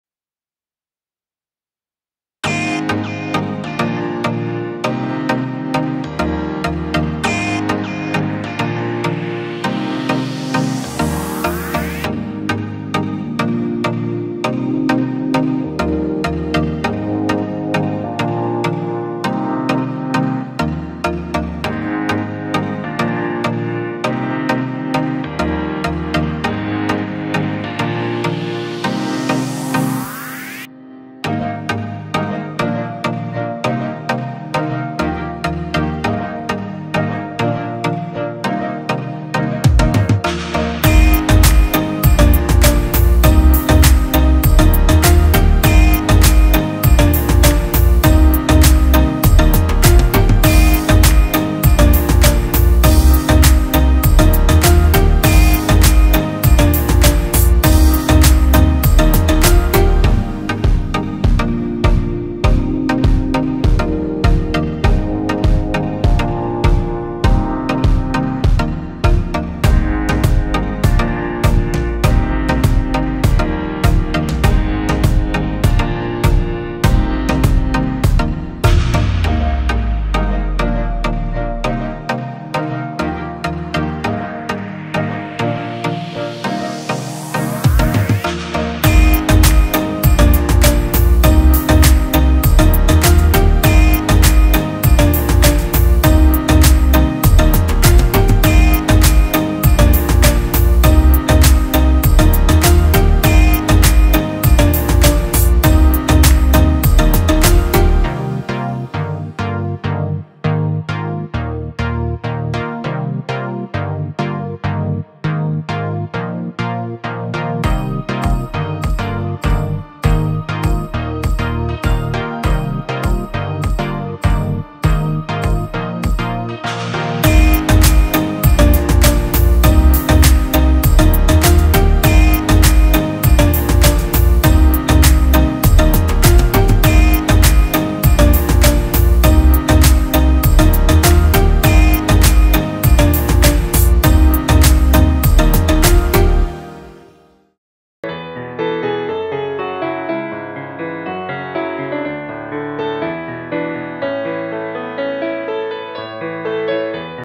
Amazing Tail Light Storage Drawers for Your Vehicle
These tail light storage drawers from Black Sheep Innovationsoffer a unique and innovative solution to the problem of limited storage space in vehicles. They are designed to fit into the space behind your tail lights, allowing you to utilize otherwise wasted space for additional storage.!😯😍❤️